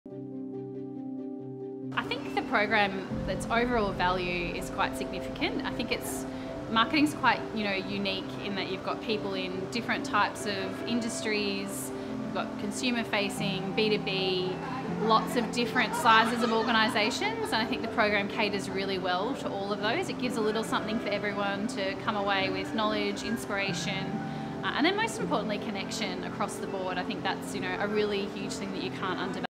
CMO ANZ Summit 2023 - Phoebe Miller from Made Group on the overall value of the Summit
marcus evans is a corporate and information company that delivers annual events across all industry sectors.